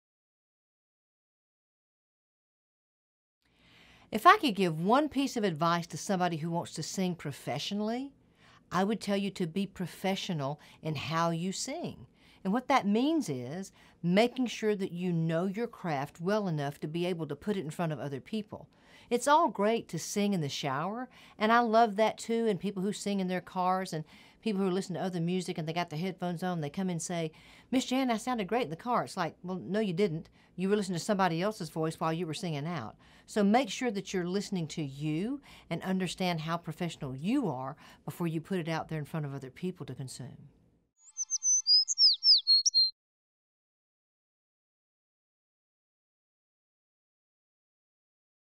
Advice for Aspiring Singers - Mama Jan Smith
If you wanna be a professional singer, here is my advice to you!